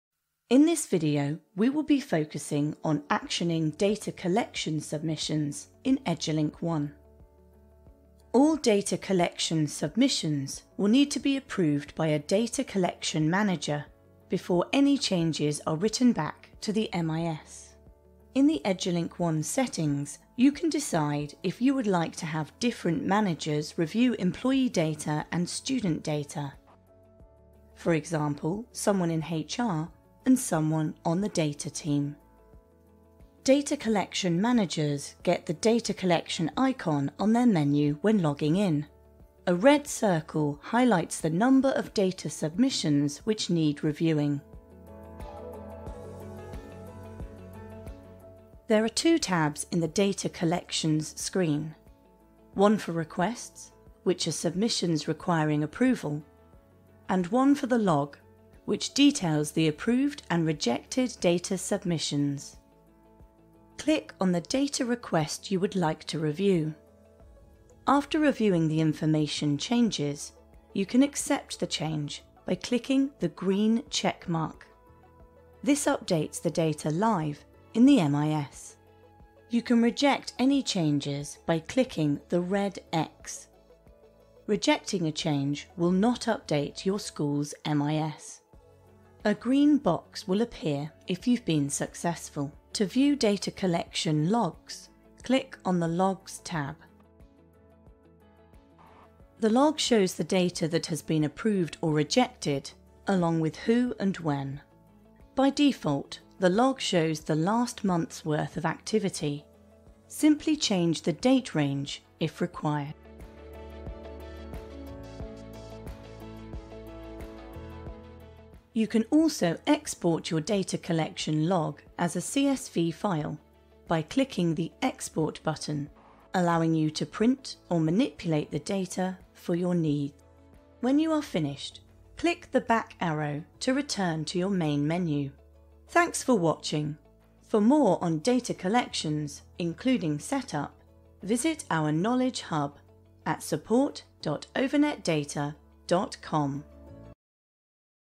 Data Collection in Edulink One
Manage your school's data collection process using Edulink One's Data Collections functionality. Administrators can accept or reject data submitted by parents, carers, learners and employees.